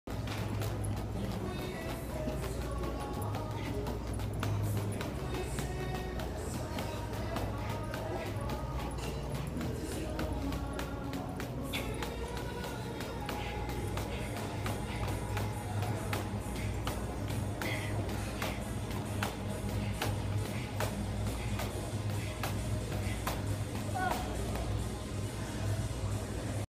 Battle Rope Tabatas
30 seconds on 30 seconds off. good burn for shoulder day.